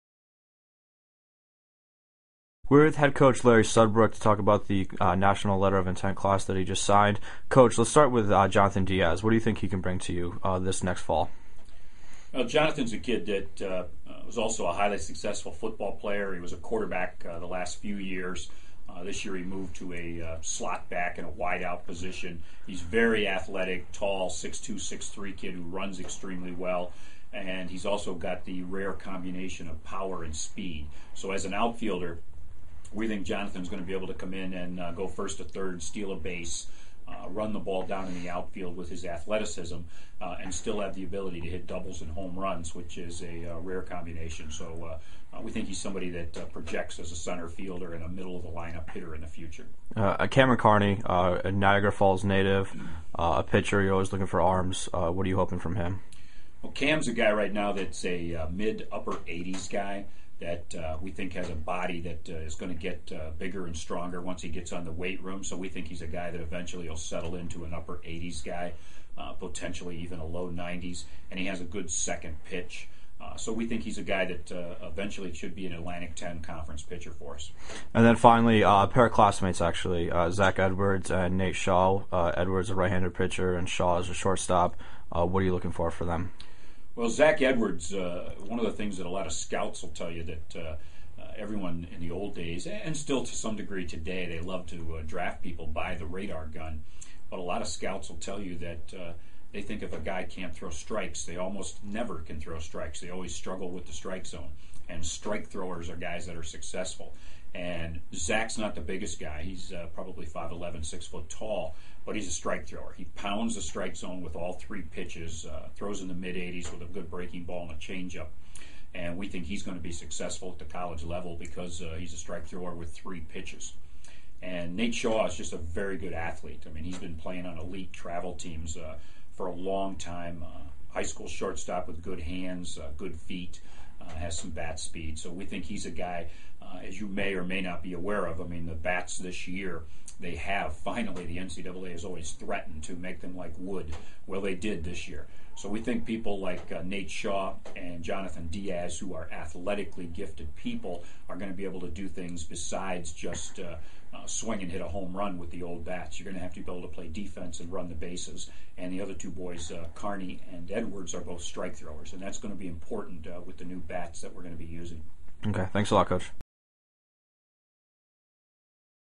Baseball: 2011-12 Recruiting Class
Head coach Larry Sudbrook discusses the early signing four-player class of Jonathan Diaz, Zachary Edwards, Cameron Carney and Nathan Shaw for the 2011-12 year.